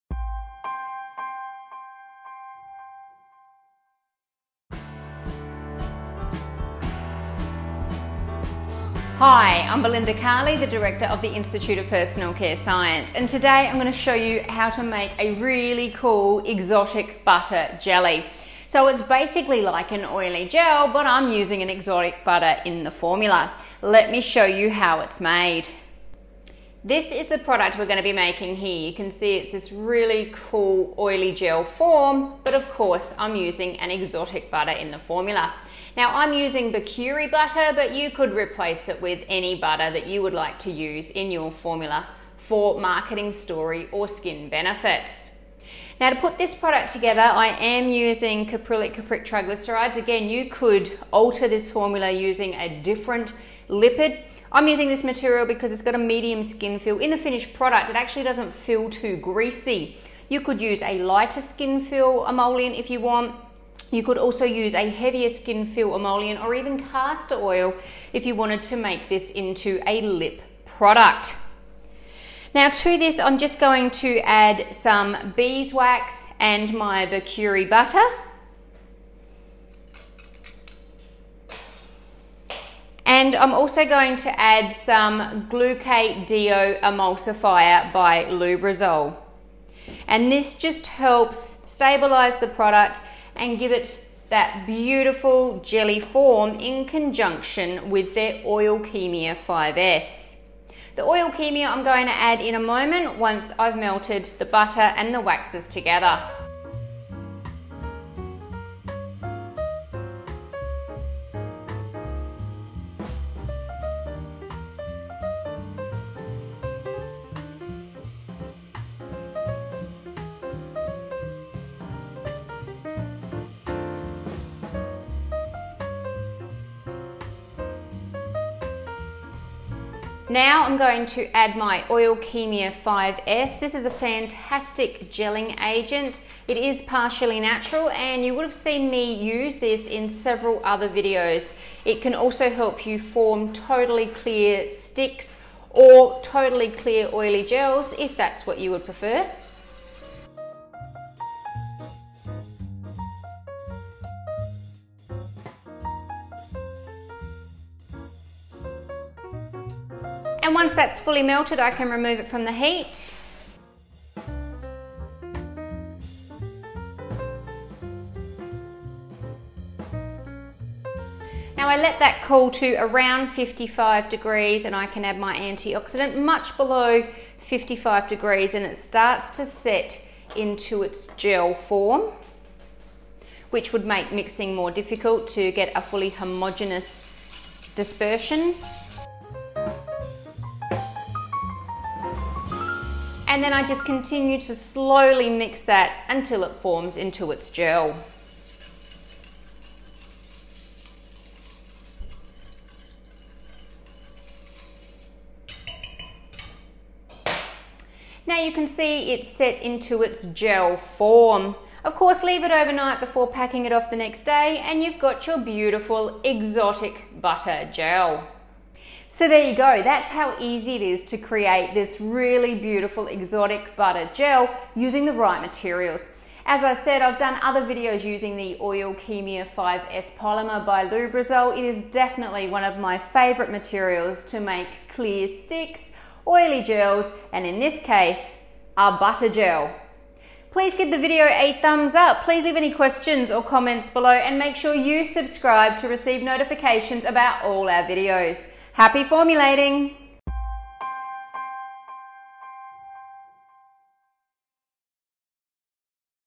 Exotic Butter Gel
Find out how to use exotic butters to create beautifully light oily gels. This oily gel is easy to make and uses bacuri butter – but you can substitute it for any other type of exotic or cosmetic butter you would like to include. Find out how, watch now!